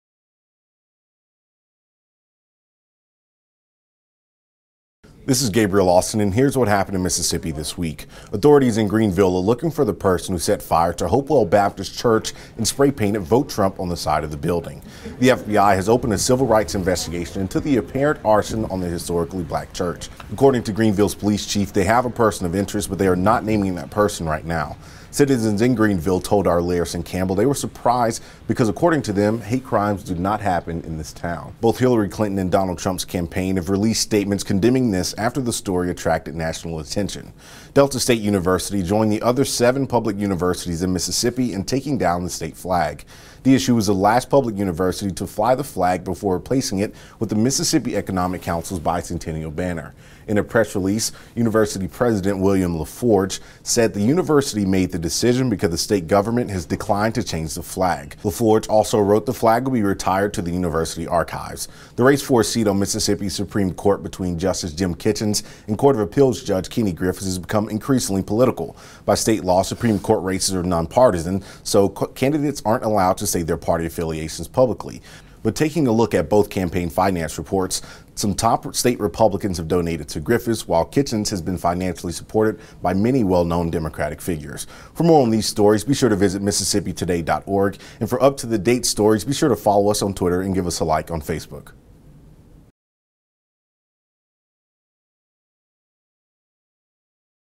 This Week: Greenville church fire, state flag down at Delta State, and political justice race
Authorities in Greenville are looking for the person who set fire to hopewell baptist church and spray painting vote trump on the side of the building.

The FBI also opened a civil rights investigation into the apparent arson on the historically black church.

According to Greenville’s police chief they have a person of interest, but they aren’t naming that person.

Citizens in Greenville told our Larrison Campbell they were surprised because according to them, hate crimes don’t happen in this town.

Both Hillary Clinton and Donald Trump’s campaign released statements condemning this after the story attracted national attention.

Delta State University joined the other seven public universities in Mississippi in taking down the Mississippi state flag.

Dsu was the last public university to fly the flag before replacing it with the Mississippi Economic Council’s bicentennial banner.

University president William Laforge said the university made the decision because the state government has declined to change the flag.

Laforge wrote the flag will be retired to the university archives.

The race for a seat on Mississippi’s Supreme Court between justice Jim Kitches and court of appeals judge Kenny Griffis has become increasingly political.

But taking a look at both campaign finance reports, some top state republicans have donated to Griffis, while Kitchens has been financially supported by many well-known democratic figures.